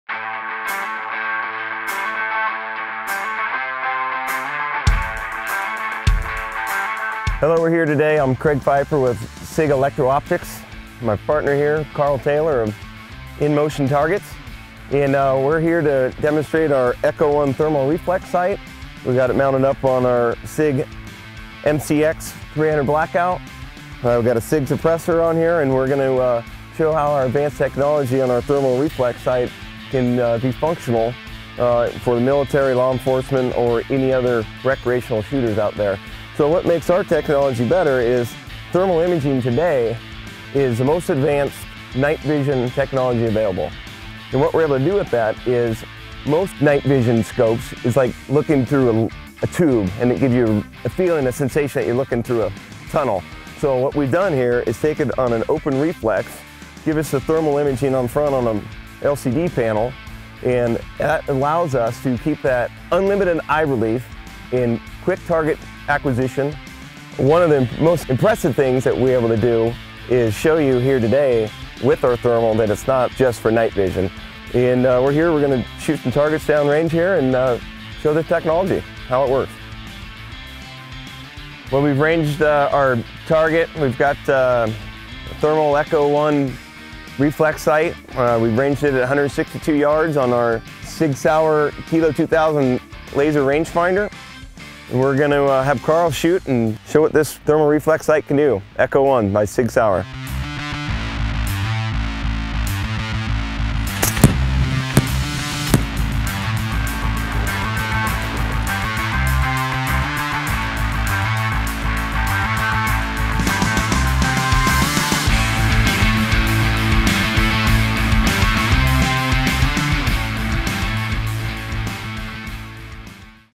SIG Electro-Optics ECHO1 Thermal Reflex Sight
Craig Pfeifer from SIG Electro-Optics and Carl Taylor from In-Motion Targets and Long Range Reality TV demonstrate the features of the ECHO1 Thermal Reflex Sight.